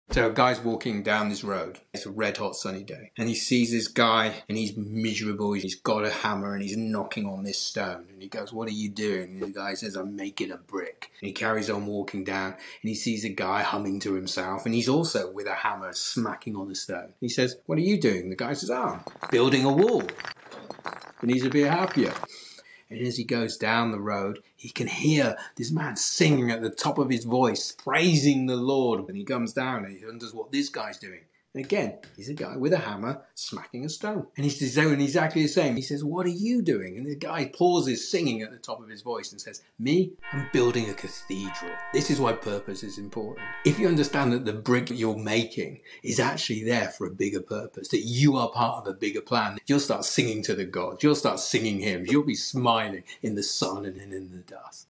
The Brick, The Wall, and The Cathedral
Here is your story of the day. Picture this:

We're strolling down a road on a sweltering day, the sun blazing down.

We spot a man, a hammer in hand, chipping away at a stone, his face a mask of misery.

We ask him, "What are you doing?" He replies, "I'm making a brick."

We continue on, and soon we come across another chap.

He's humming a tune, rhythmically tapping away at his stone with a hammer.

We ask the same question, "What are you doing?"

He says, "I'm building a wall."

As we walk further down the road, we suddenly hear singing at the top of his voice, praising the Lord.

Once again, we find a man with a hammer, striking a stone.

However, his demeanour is different.

He's positively radiant, his joy infectious.

We ask him, "What on earth are you doing?"

He pauses his singing, grins at us, and declares, "I'm building a cathedral."

This story is a reminder of the power of purpose.

If we comprehend that the seemingly insignificant tasks we undertake are, in fact, part of a grander scheme, our perspective shifts drastically.

Suddenly, every brick we shape is a step towards creating a masterpiece.

We're no longer just bricklayers, but architects of cathedrals.

This understanding can transform our attitude, turning our toil into a song, a hymn of joy, a testament to our purpose.

So, the next time we find ourselves in the scorching sun or amidst the dust, remember this: we're not just creating bricks, we're building cathedrals.

Want to work together? Check me out at Rhino Consulting